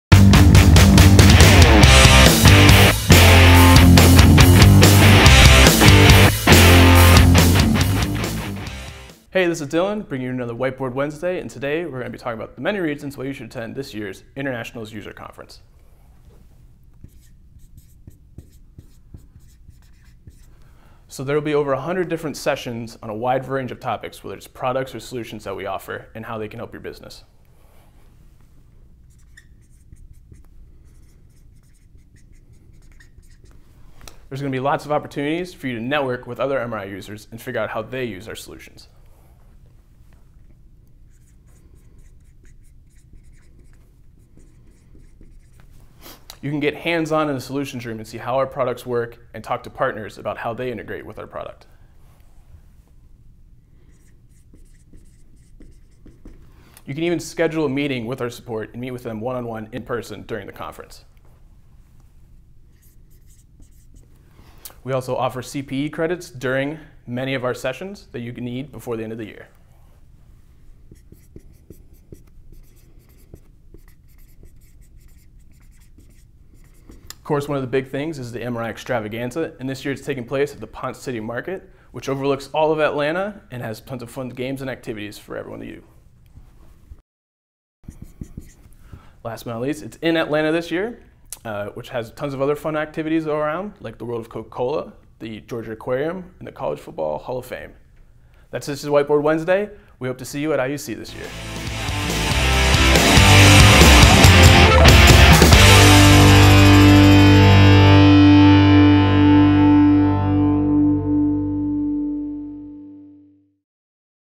Whiteboard Wednesday - 27 - Why YOU should attend IUC 2018
On this episode of Whiteboard Wednesday, Dylan Moats explains many of the reasons you should attend this year's MRI International Users Conference in Atlanta Georgia, October 14-17.